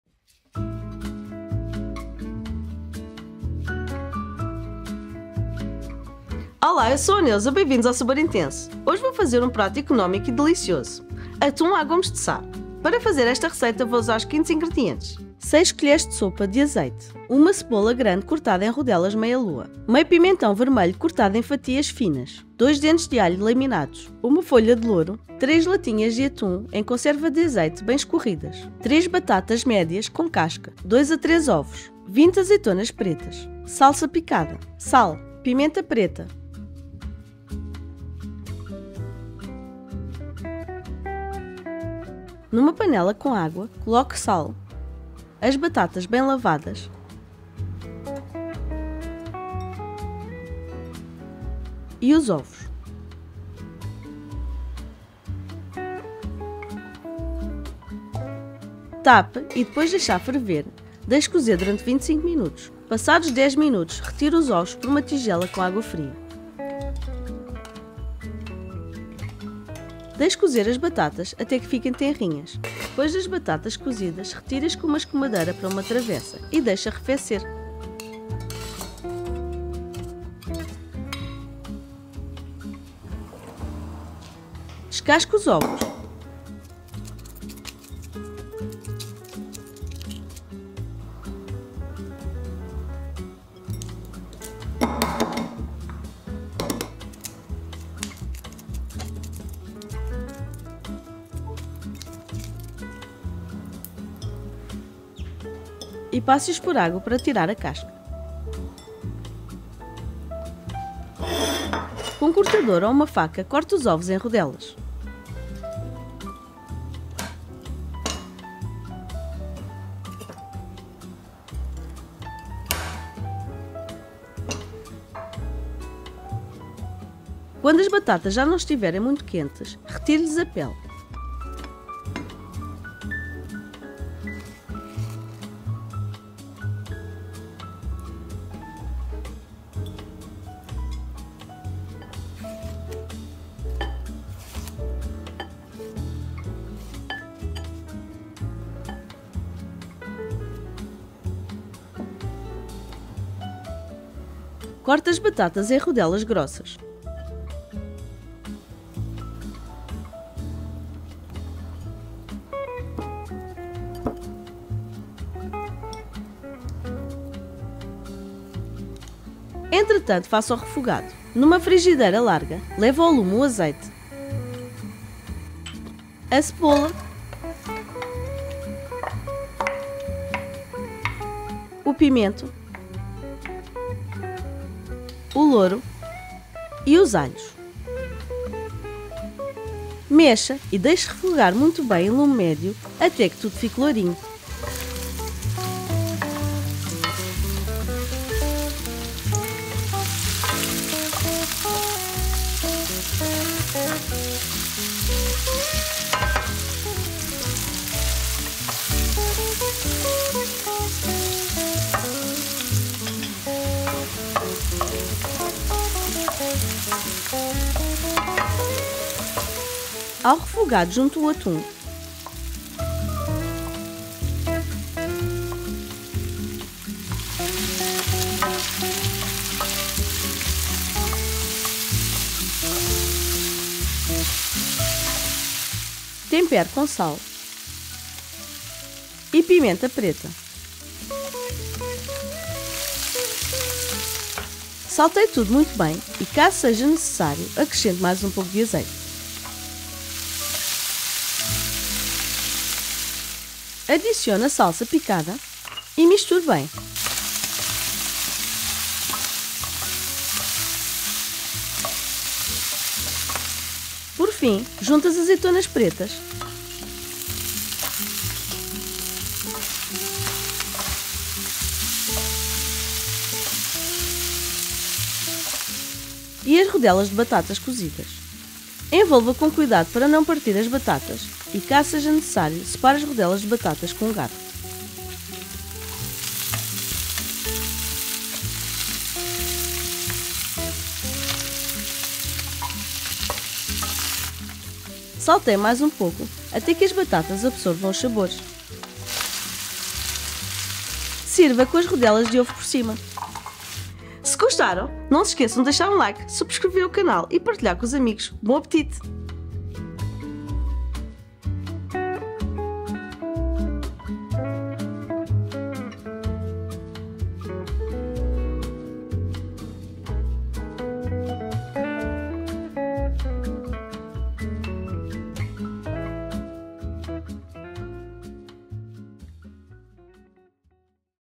Atum à Gomes de Sá
Como cozinhar a receita de Atum à Gomes de Sá

👩 Acerca da receita: Esta receita é a versão do Bacalhau à Gomes de Sá mas feita com Atum.
É uma versão mais económica e rápida que a original mas igualmente deliciosa.
Se quiserem podem fazer com outra conserva de atum a gosto ou até atum fresco fritando-o primeiro e depois desfiando-o.
Espero que gostem, bom apetite!
💚 Neuza Costa 💚

📋 Ingredientes para a receita:

6 colheres de sopa de azeite
1 cebola grande cortada em rodelas meia lua
½ pimentão vermelho cortado em fatias finas
2 dentes de alho laminados
1 folha de louro
3 latinhas de atum em conserva de azeite bem escorridas
3 batatas médias com casca
2 a 3 ovos
20 azeitonas pretas
Salsa picada
Sal
Pimenta preta

🍴 Para 3 pessoas
⏳ Tempo de preparação: 45 minutos

★ SEGUE-NOS PARA RECEITAS TODOS OS DIAS ★